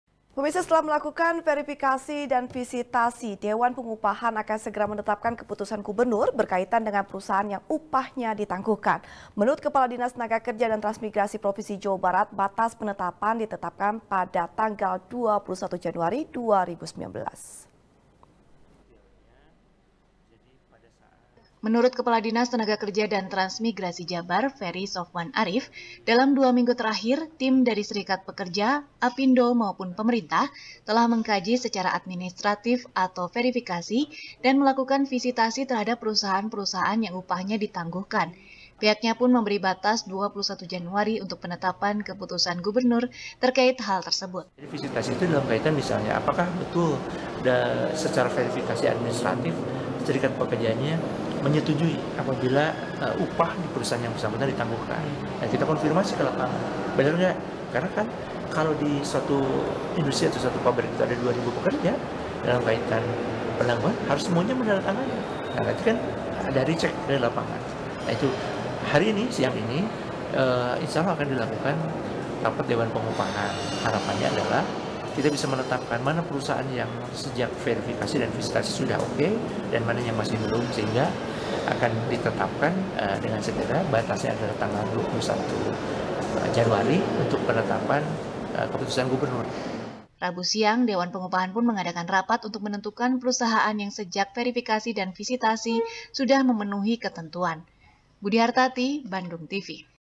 Upah Ditangguhkan, 21 Januari Batas Keputusan Gubernur | SBR | BANDUNGTV
SBR | BANDUNGTV | FARHAT MUMTAZ | Setelah melakukan verifikasi dan visitasi, dewan pengupahan akan segera menetapkan keputusan gubernur, berkaitan dengan perusahaan yang upahnya ditangguhkan. menurut kepala dinas tenaga kerja dan transmigrasi jabar, batas penetapan ditetapkan pada 21 januari 2019.